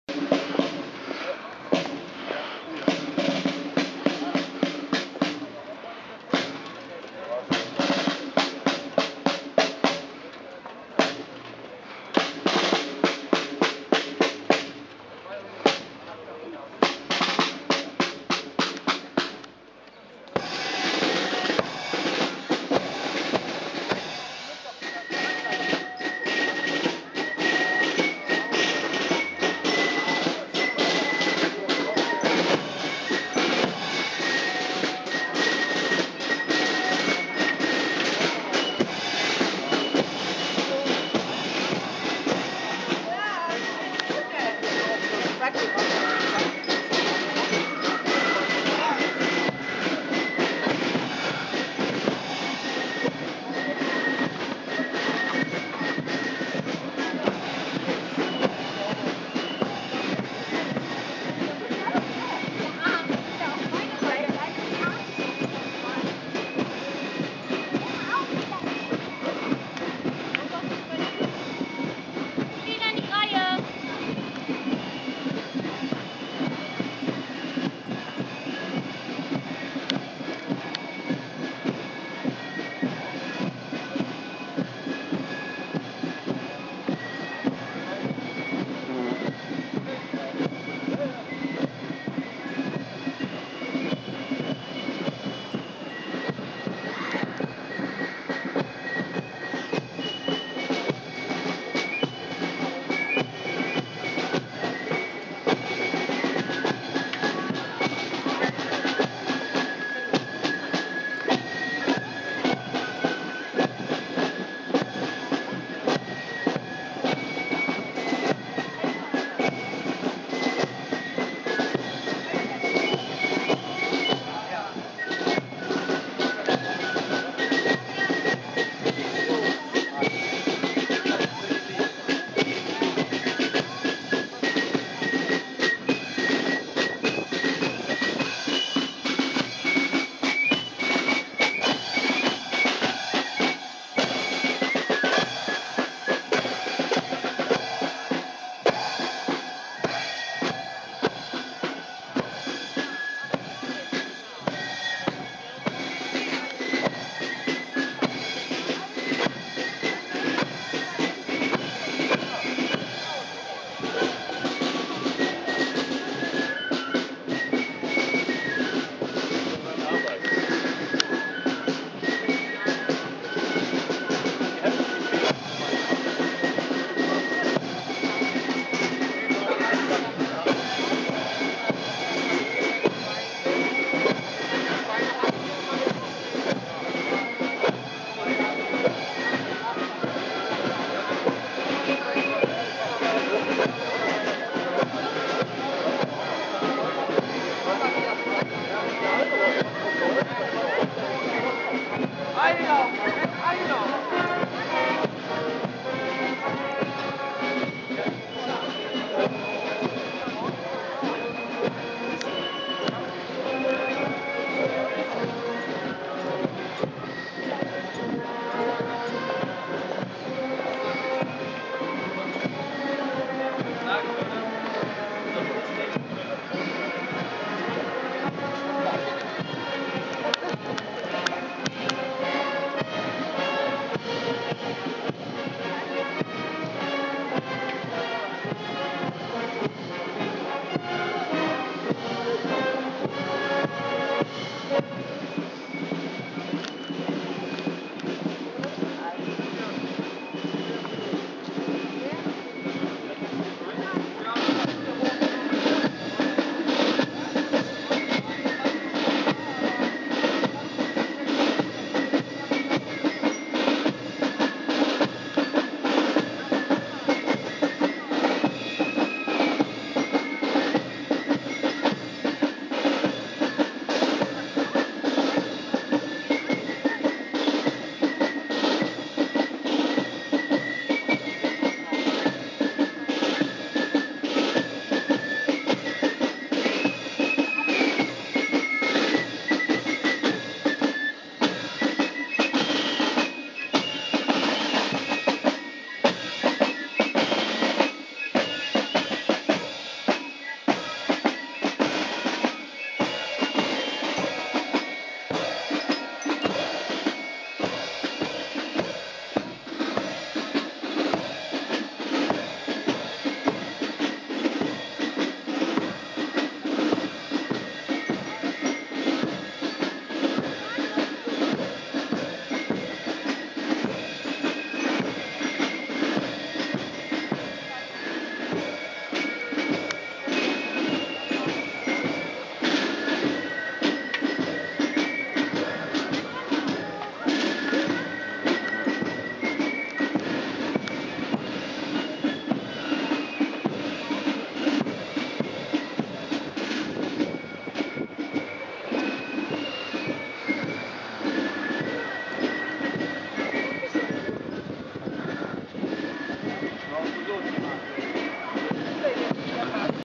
Schützenfest Porz Urbach Köln 2013
Der Festzug zieht durch die Fauststraße,